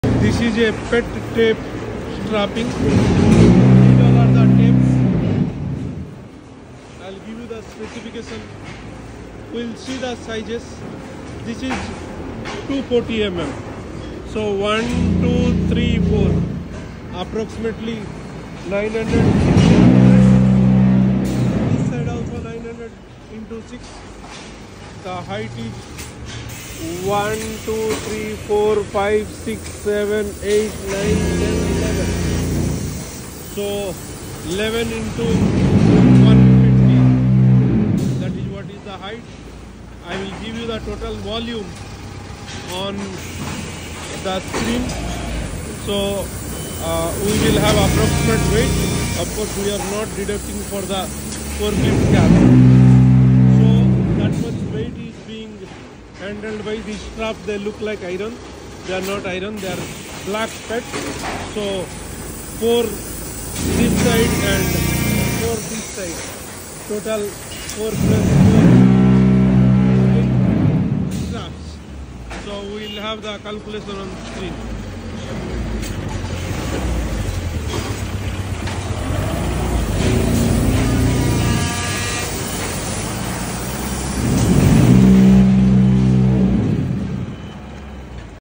Efficient Automatic Packing Line for Brickmaking | No Palette Packing & Stacking || Dongyue Group
Discover the benefits of the Don Automatic Packing Line for brickmaking machines! This advanced system eliminates the need for palettes, making packing and stacking more efficient.

Key Highlights:

✅ No Palette Required – Ensures seamless stacking and transport
✅ Forklift-Friendly Design – Each brick stack includes grip holes for easy lifting and transport
✅ Optimal Load Distribution – Secure stacking for safer handling

Stock Weight & Strap Holding Capacity Analysis:

📌 Brick Stack Dimensions: 960mm × 960mm × 1265mm
📌 Brick Density: 1750 kg/m³
📌 Total Stock Weight: 1816 kg

To securely hold the stack, eight PET straps are used, each with a holding capacity of 300 kg, providing a total holding strength of 2400 kg—well above the required weight.

Ensuring Safety & Quality:

The most crucial factor is the holding capacity of the strap welder joint. Thanks to Dongyue’s high-quality machines, we ensure precise and reliable welding, making every stack safe for handling and transportation.